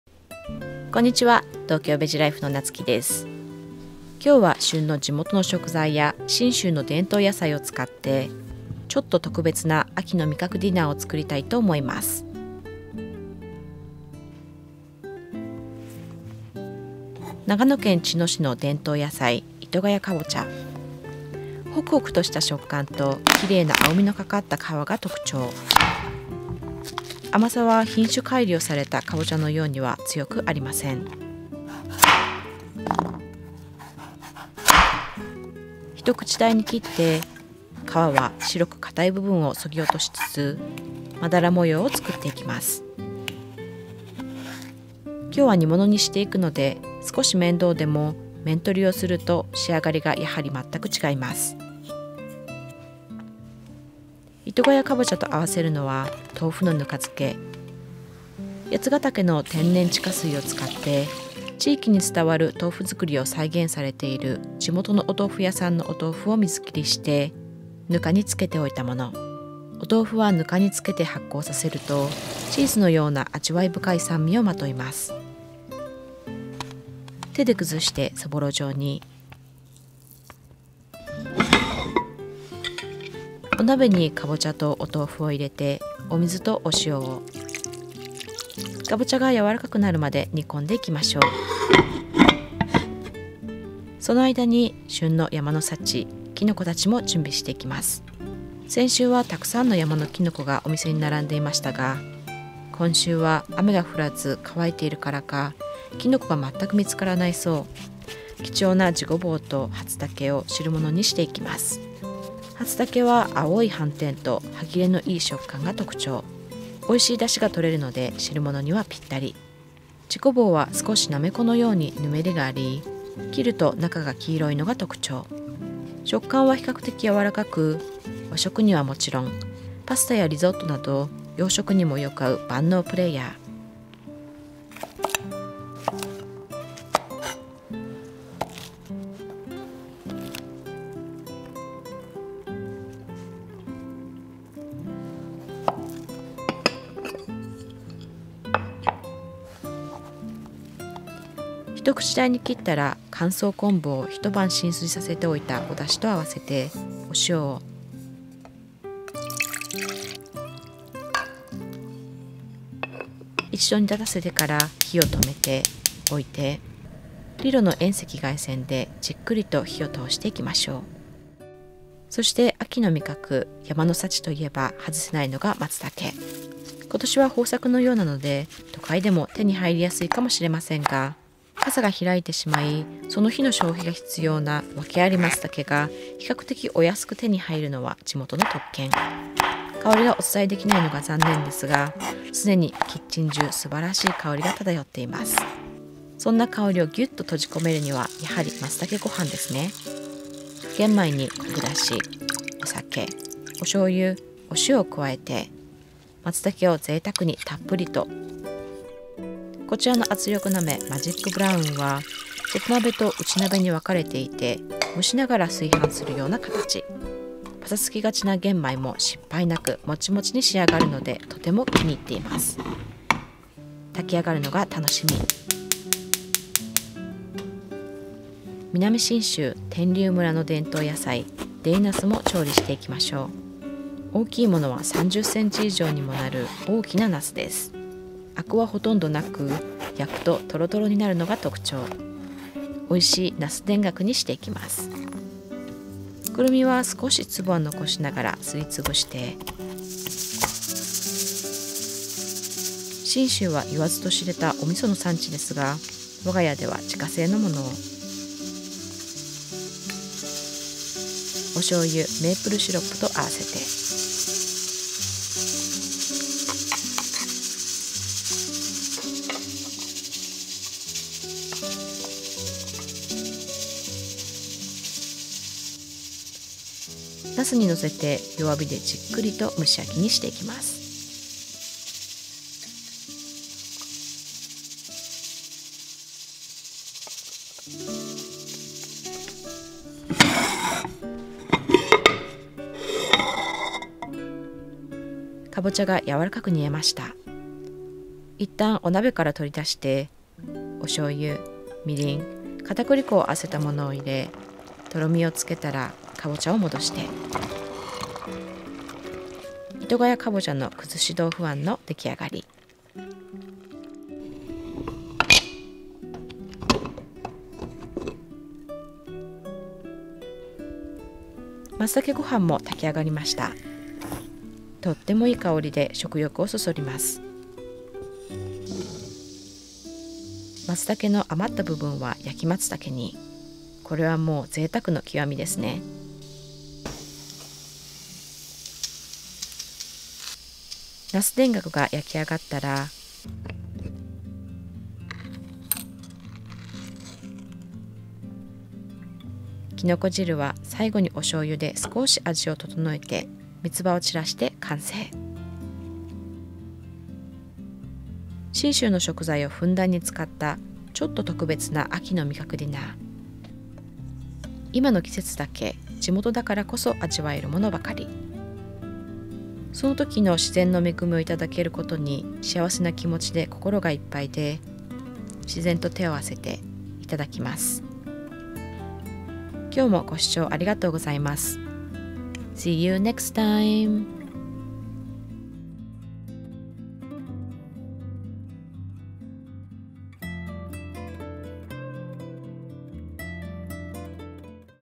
信州の秋の味覚和食ディナー☆松茸、山のきのこ、糸萱かぼちゃ、でいなす
信州の旬の食材、伝統野菜を使って、秋の味覚和食ディナーを作っていきます。

使用している調理器具
liloダッチオーブン

圧力鍋 マジックブラウン

長谷園 ふっくらさん 土鍋

***TOKYO VEG LIFE初出版のレシピ本絶賛販売中！！！***
「おうちでヴィーガン 14日間で旅する世界一周の味」